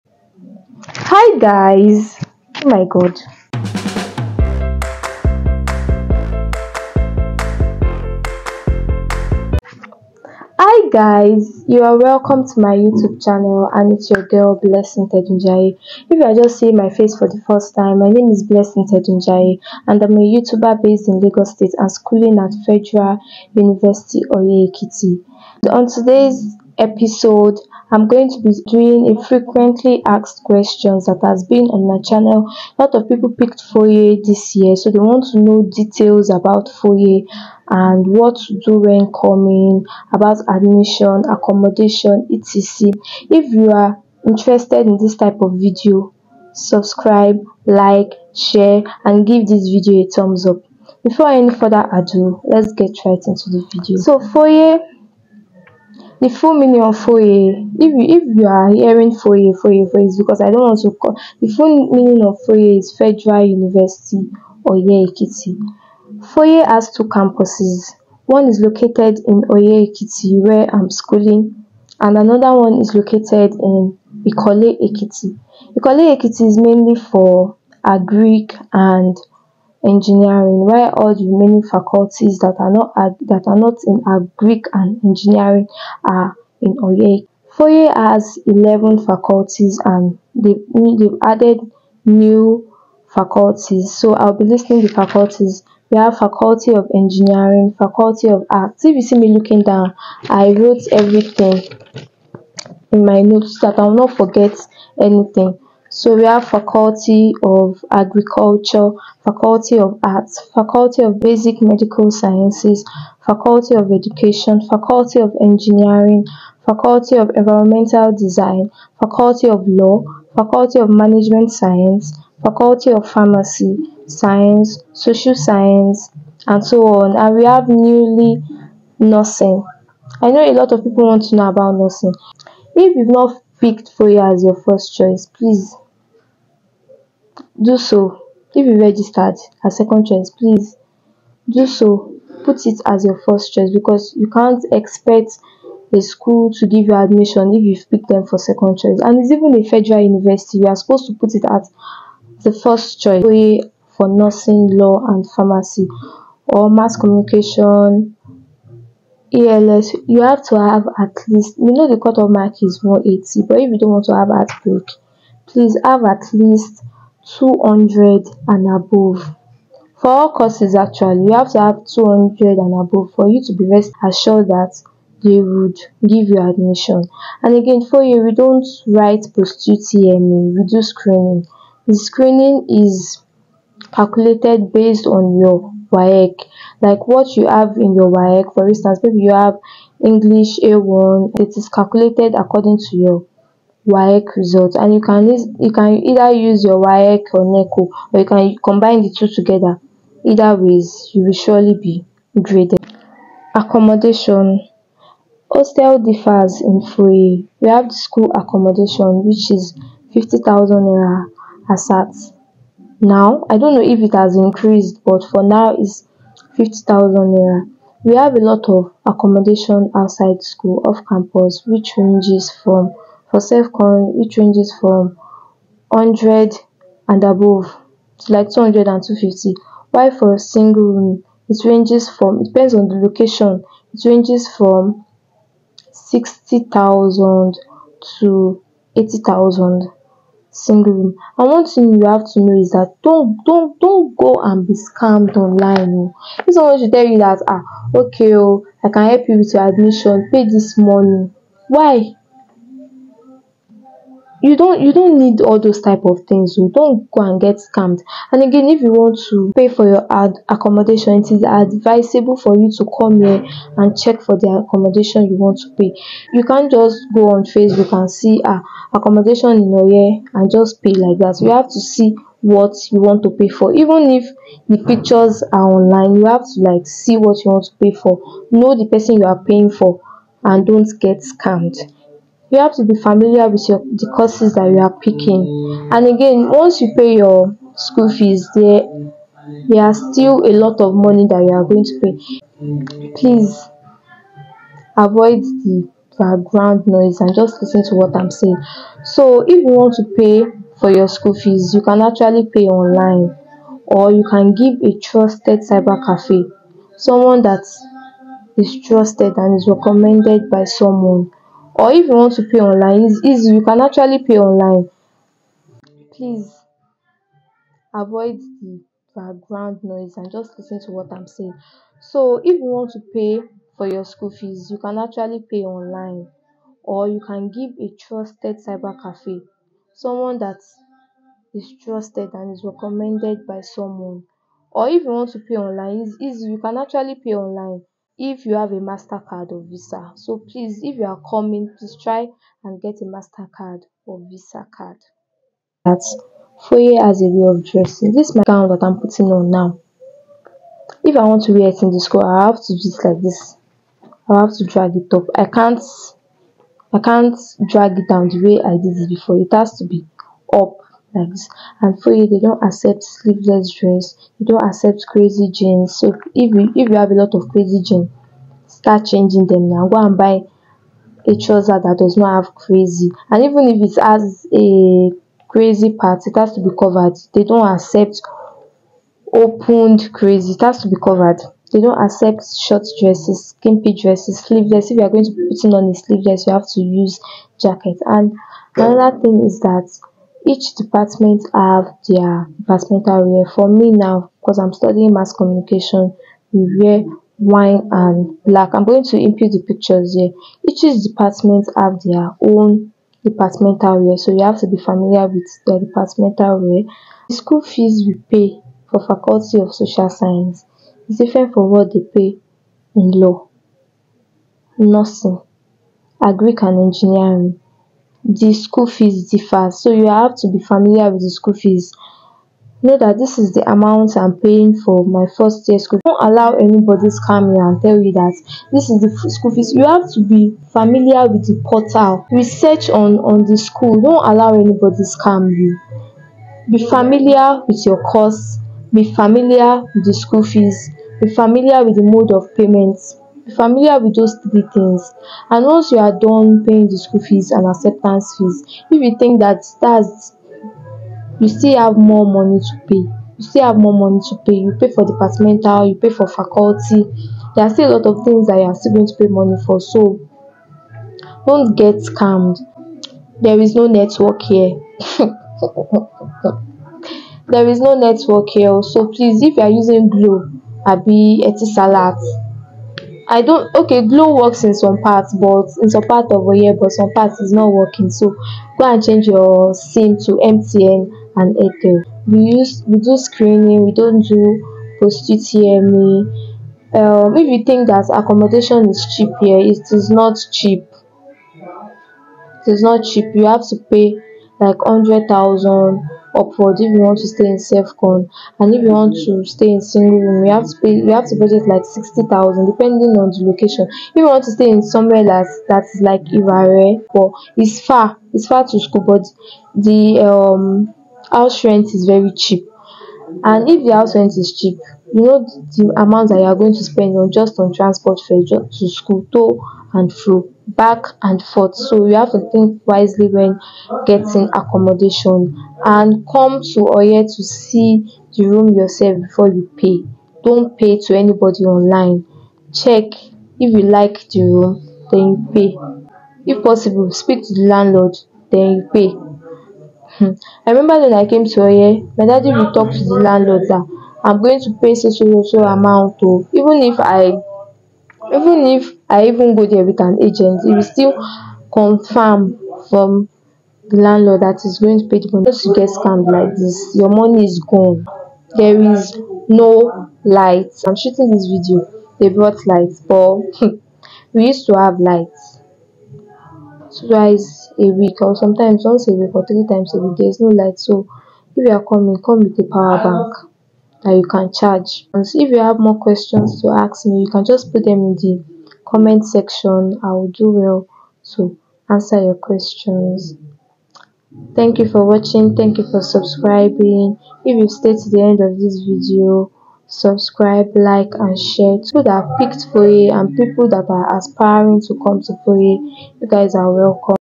What you need to know about Federal University Oye Ekiti
Watch in 1080hd for good quality… I shot this video in the evening
And I also have other things to talk about which would be in another video, I did not want this video to be unnecessarily long that was why I made it short
If there are other things you want me to talk about you can let me know it in comment section